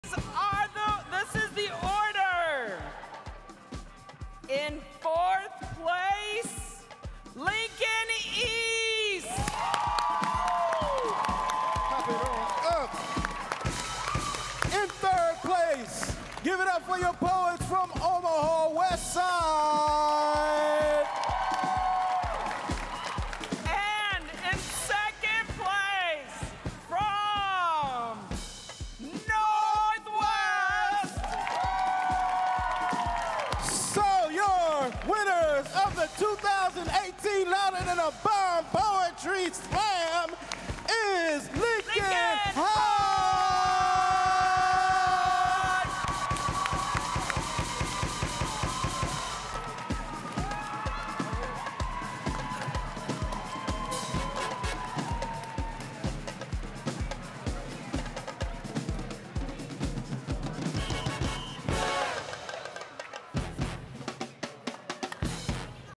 Louder Than a Bomb Great Plains, 2018 Finals
Louder Than a Bomb Great Plains, 2018 Finals April 24th at the Holland Center for Performing Arts.

Featuring teams from: Lincoln High School, Omaha Northwest, Westside and Lincoln East.